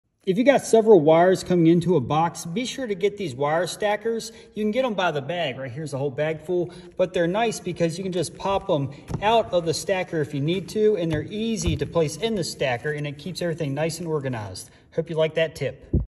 How To Use Wire Stackers!
When I wire a house I like to use wire stackers. This is how to use wire stackers!